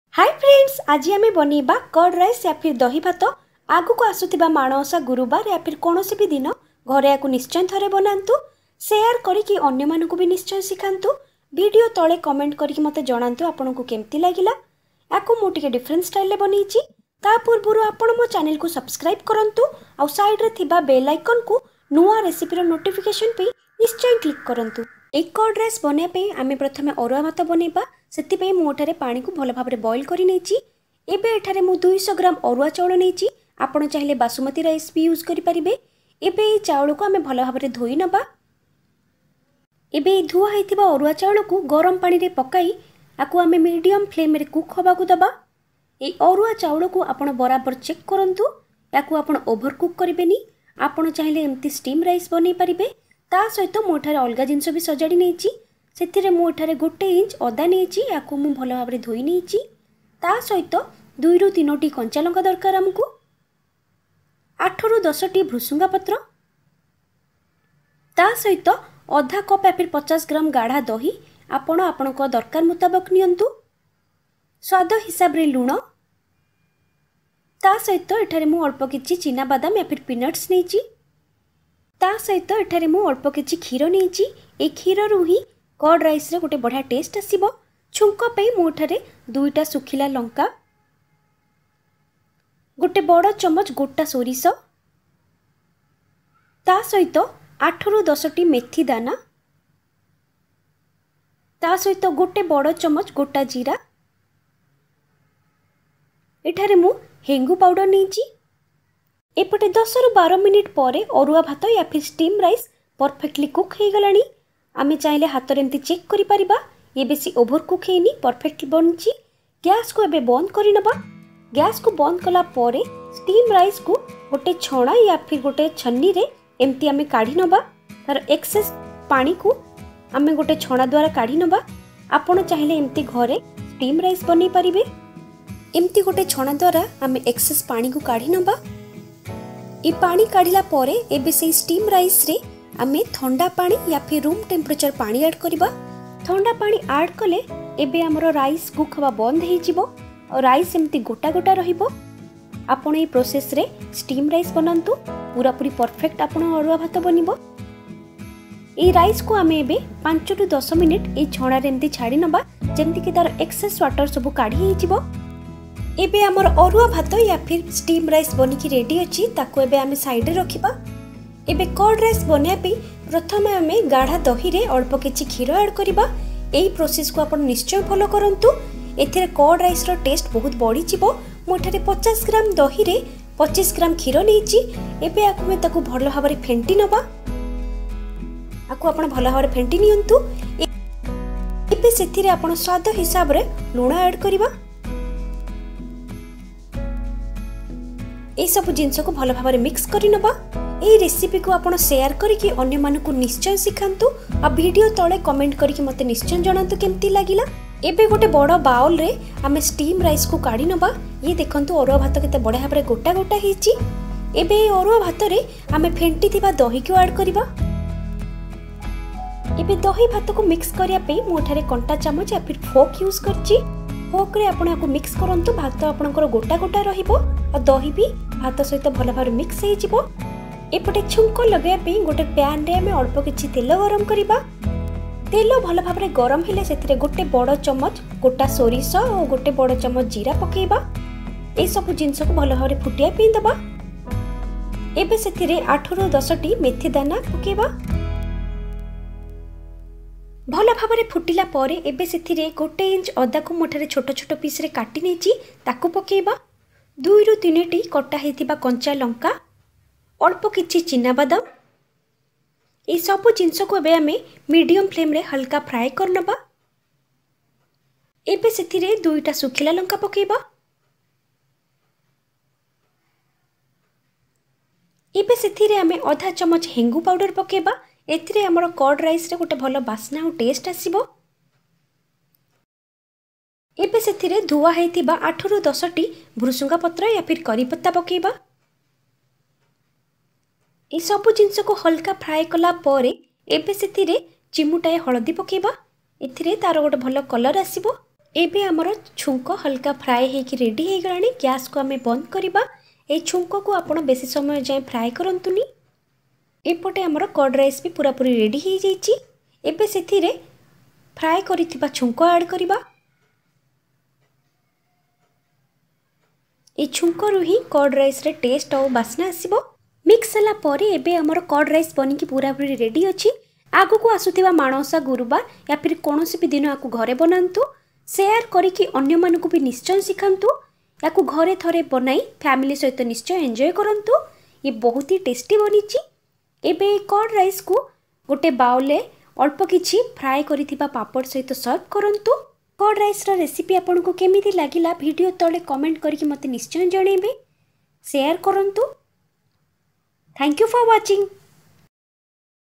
Curd Rice | ଦହି ଭାତ ମାଣବସା ଗୁରୁବାର Spl |Delicious Lunch Recipe wd English Subtitles
Learn how to make Curd Rice Step by step. It's a simple n Delicious Recipe which is very popular all over India. Perfect for LunchIdeas n HealthyRecipe.. SHARETheRecipe wd Others.

Ingredients:
White Rice /Basmati Rice
Curd
Milk
Chopped Ginger
Green Chilly
Curry Leaves
Peanuts
Mustard Seeds
Cumin Seeds
Methi/Fenugreek Seeds
Dry Chillies
Hing/Asafoetida powder
Turmeric Powder
Salt To Taste
Oil
Prepare @Home n Let me know In Comment Section Below.